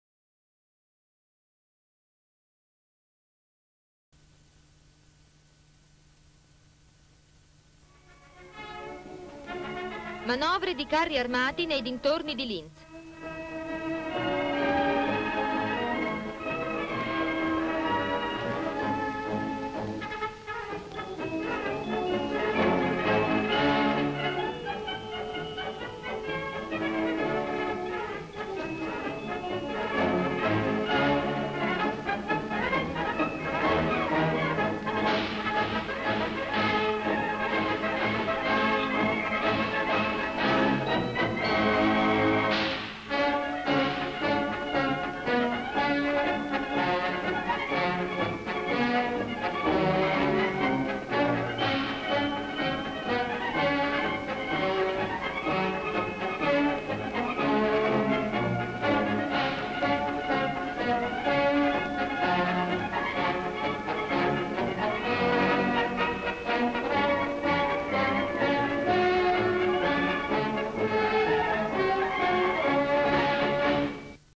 Esercitazioni di carri armati nelle campagne di Linz.
Descrizione sequenze:sfilano carri armati per le vie di Linz ; la gente presente alla sfilata dei carri armati ; le manovre dei carri armati si spostano nelle campagne di Linz ; gli spettatori ; i carri armati si esercitano su vari tipi di pendii ;